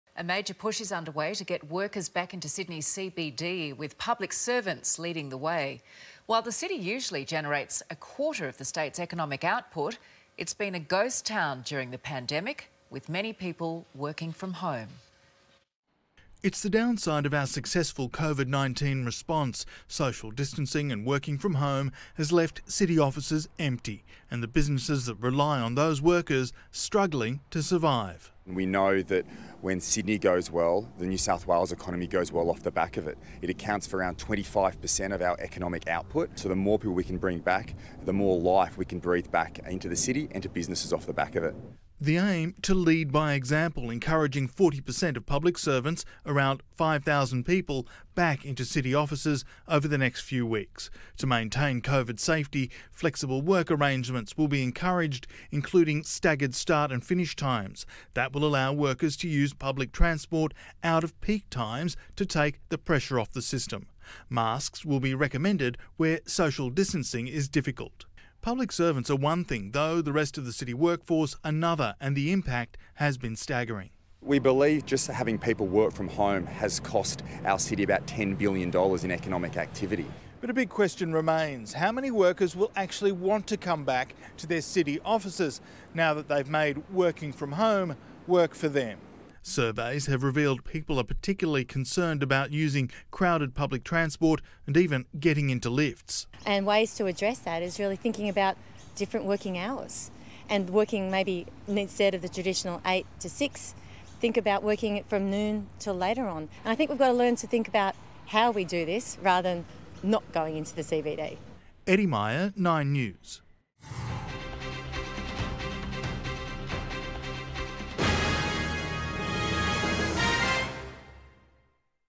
Coronavirus: Push for workers to return into offices | 9 News Australia
A major push is underway to get workers back into the Sydney CBD over the next few weeks, in a safe transition from working from home to being back in the office.